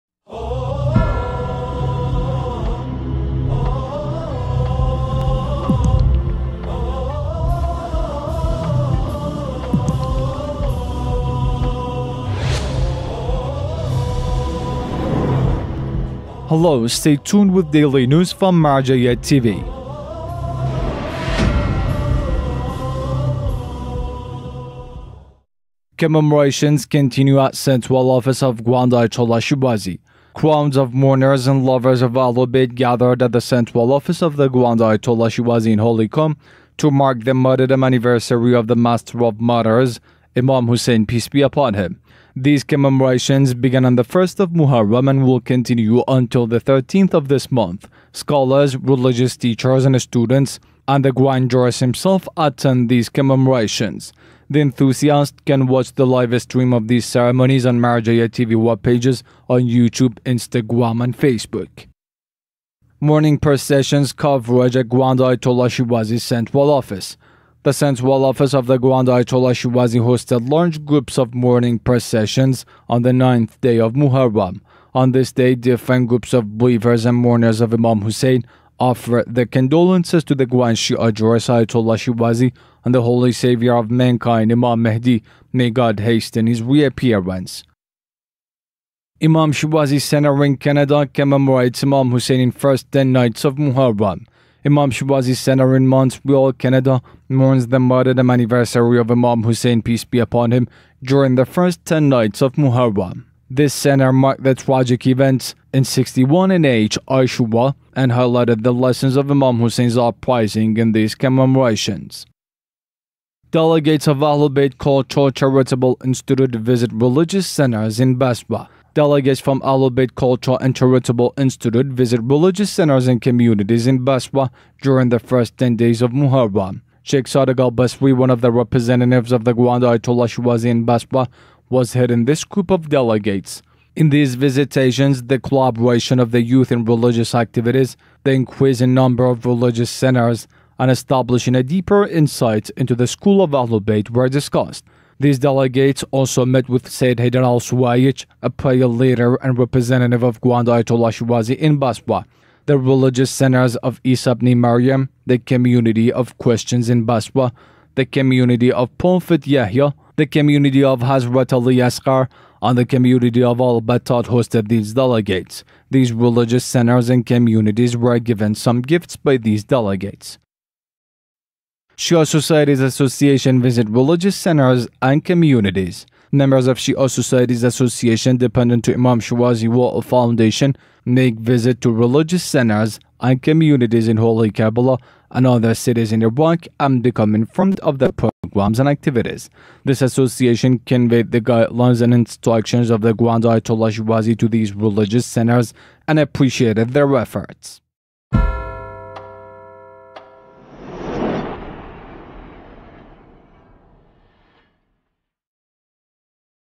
News & Events Muharram10 th 1439
🔸Commemorations Continue at Central Office of Grand Ayatollah Shirazi
🔸 Mourning Processions Converge at Grand Ayatollah Shirazi’s Central Office
🔸Imam Shirazi Center in Canada Commemorates Imam Husayn in First Ten Nights of Muharram
🔸Delegates of AhlulBayt Cultural-Charitable Institute Visit Religious Centers in Basra
🔸Shia Societies Association Visit Religious Centers and Communities